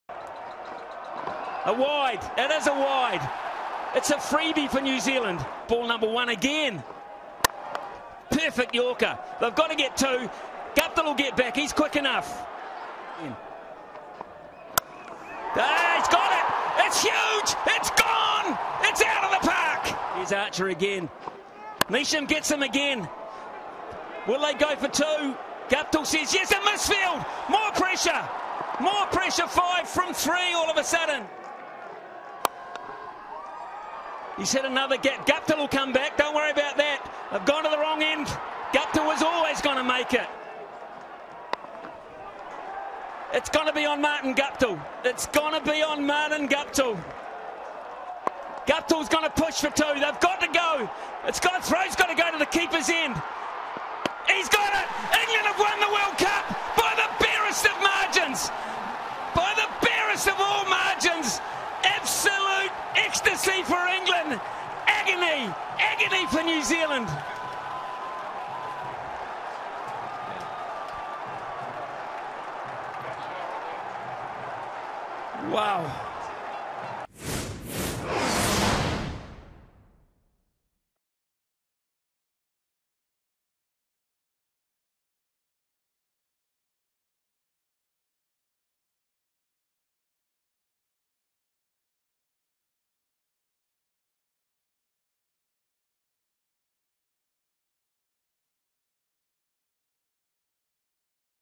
New Zealand Super Over | Every Ball | ICC Cricket World Cup 2019
Watch every ball of New Zealand's Super Over in the ICC Men's Cricket World Cup final 2019 vs England.